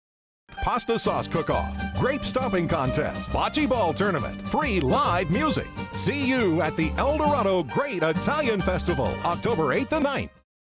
The Great Italian Festival 2011
It's a festival frenzy of Italian food, music and fun at The Eldorado Great Italian Festival, Saturday, October 8 from 10am to 7pm and Sunday, October 9 from 10am to 6pm. The 30th annual festival features the sights, sounds and tastes of Italy all day on Virginia Street in front of the Eldorado Hotel Casino.